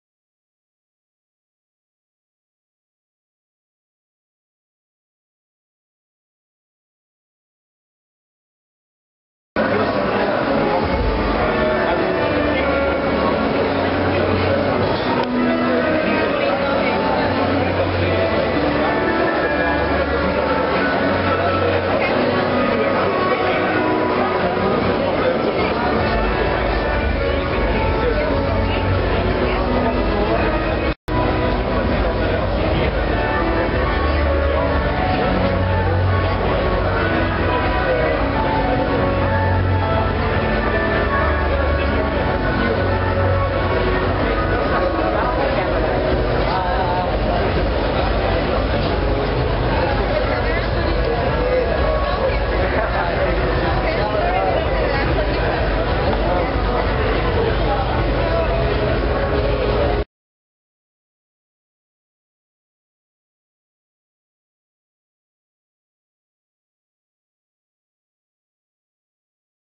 Silvia Saint signing at Antwerp Eroshow 20091003
Silvia Saint signing her picture for fans at the Antwerp Eroshow on Saturdaynight 3 October 2009.